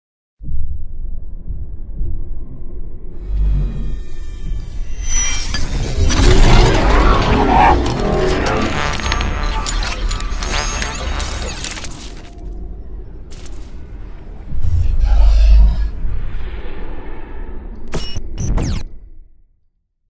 sigla 3D corporate
esempio di sigla/intro per siti web, video aziendali-corporate